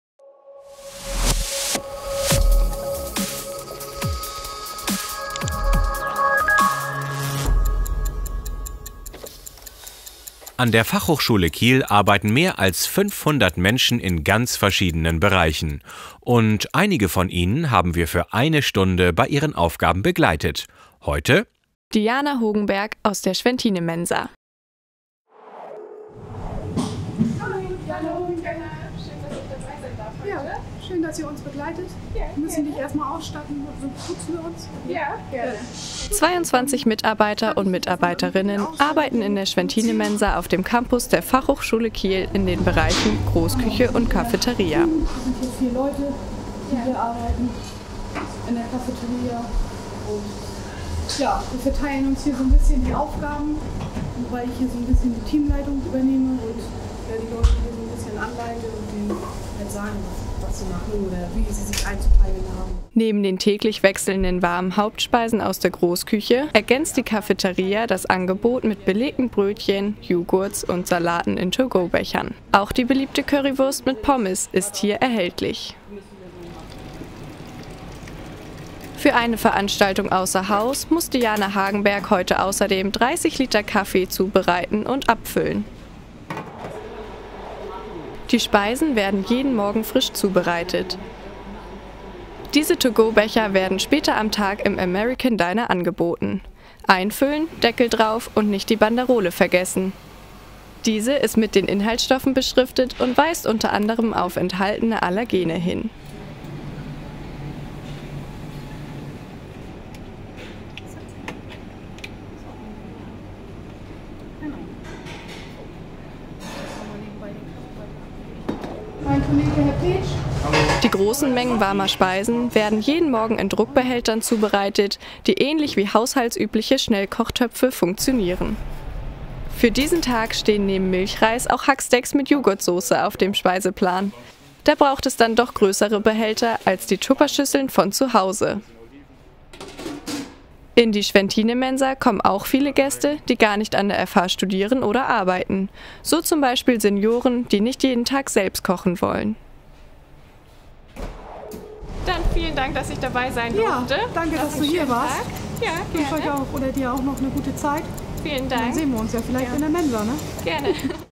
Eine Stunde mit - Diana Hoogenberg
An der Fachhochschule Kiel arbeiten mehr als 500 Menschen in ganz verschiedenen Bereichen. Einige von ihnen, haben wir für eine Stunde bei ihren Aufgaben begleitet.
Heute: Diana Hoogenberg aus der Schwentinemensa.

Ein Beitrag von Jönna Hirsch, Martje Nehlsen und Lea Wiegand.